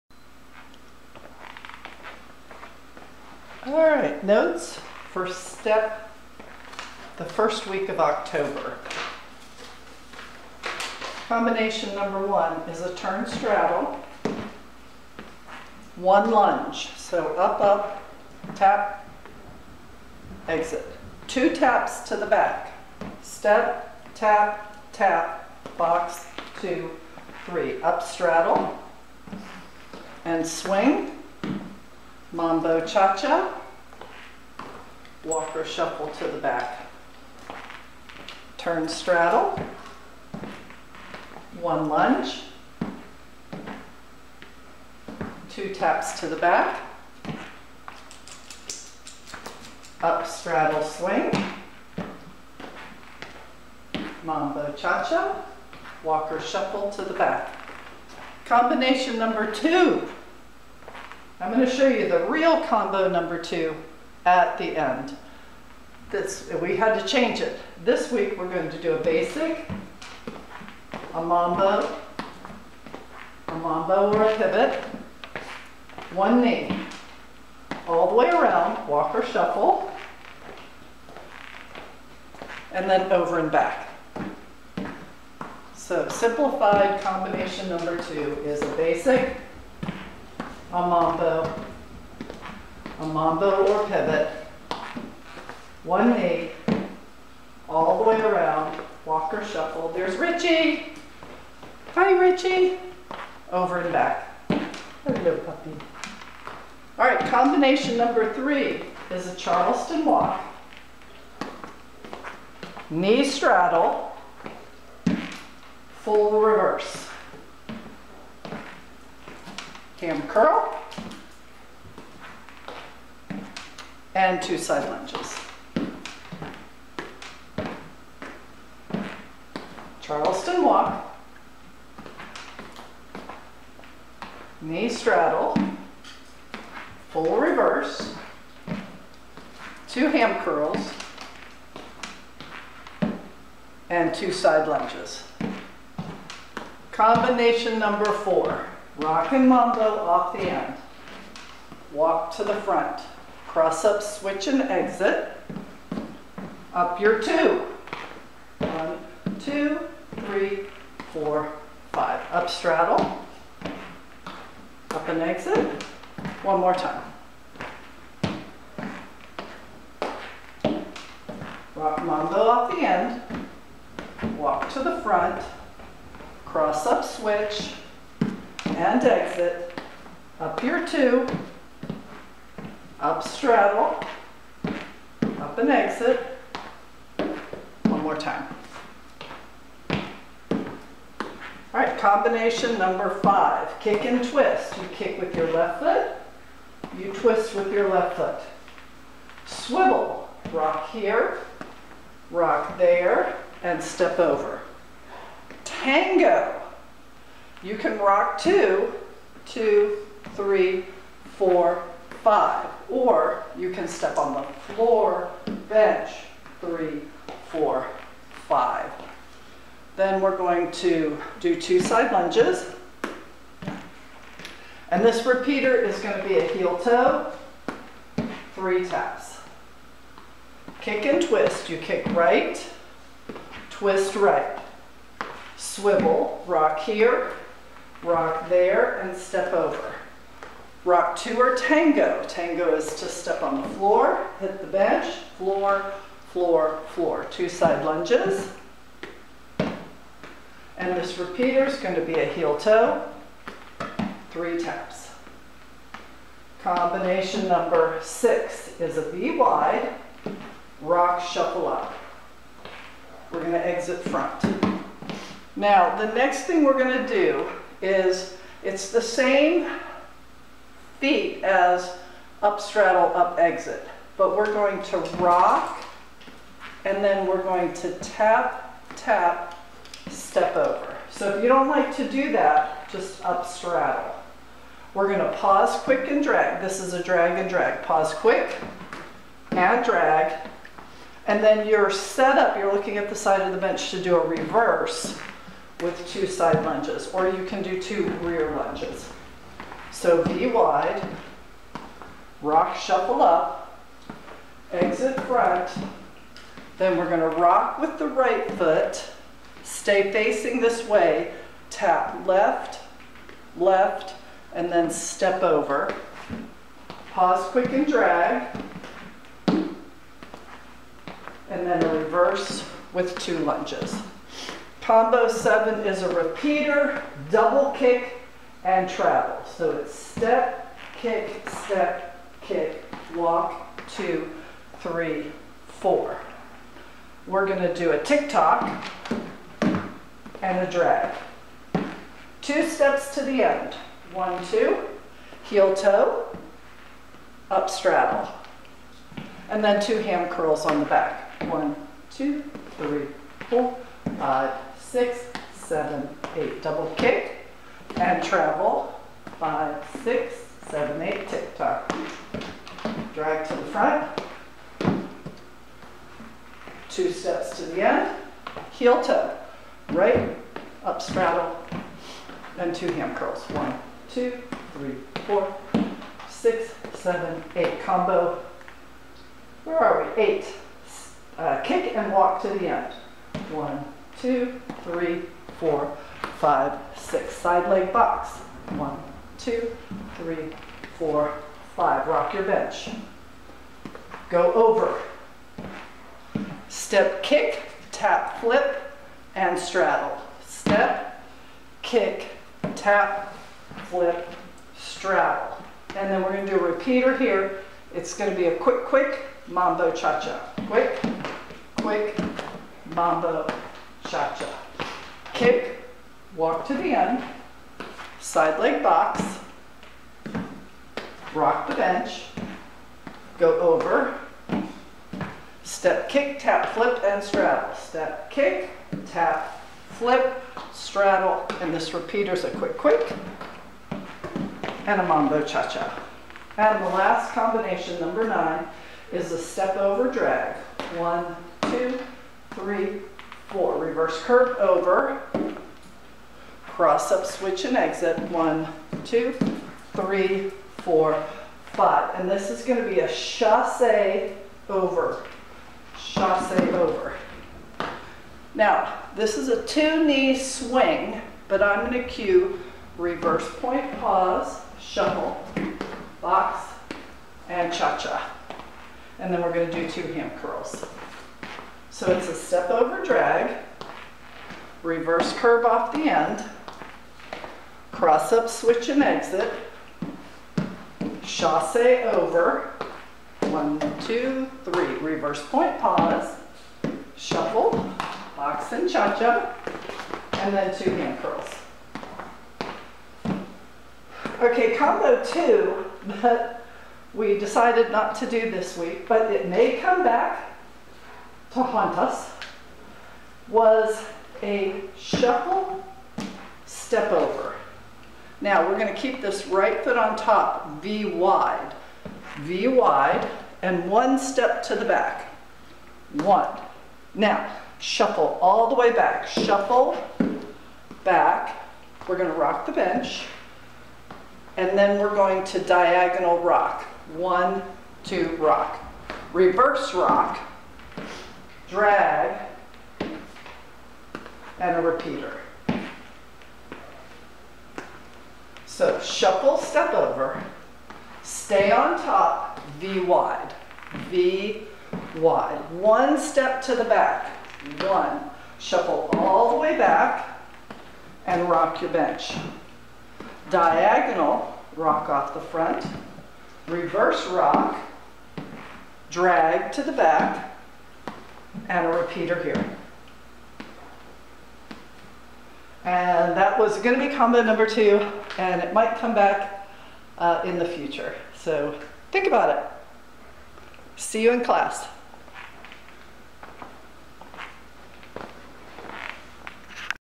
Notes for Step 10 6,8 2022
1. turn straddle - one lunge - 2 taps to the back - up straddle, swing - mambo cha cha walk to the back
2. basic - mambo - mambo/pivot - knee - all the way around - over & back
3. Charleston walk - knee straddle, full reverse - ham curl (2) - 2 side lunges
4. L step, rock mambo walk to front - cross up switch exit - up your 2 - up straddle (2)
5. kick & twist - swivel, tango - 2 side lunges - repeater (heel, toe, 3 taps)
6. V wide, rock, shuffle up, exit front - rock the bench, same side tap bench, tap floor, step over - pause quick & drag - reverse 2 lunges
7. repeater (double kick, travel) - tic toc, drag - 2 steps to the end, heel/toe - up straddle - ham curl (2)
8. kick and walk to the end 3456 - side leg 78 - box on top 1, exit 23, rock 456 - over 7812 - step kick tap flip 3456, straddle 78 - repeater (quick, quick, mambo cha cha)
9. step over drag - step off the end/over - cross up switch exit, chasse over - reverse point pause and shuffle, box and cha cha - ham curl (2)